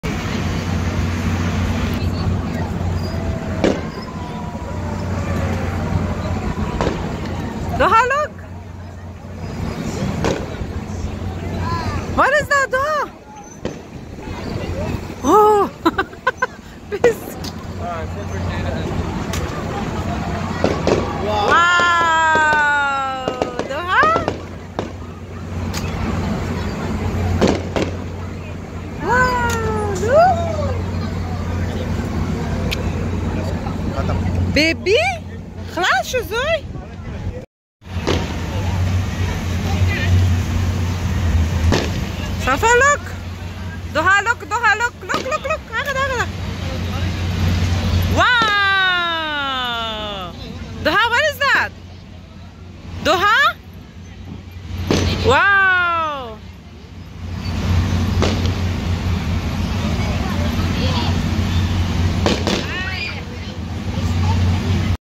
fireworks 4th of july2022
Welcome to Duha shah channel - where duha and her brother asar play, learn and share their life experiences.
In this video duha ,and asar went to the fireworks for the first and she was so surprised by seeing the fireworks .so duha have more good things to share so stay connected and support duha shah channel.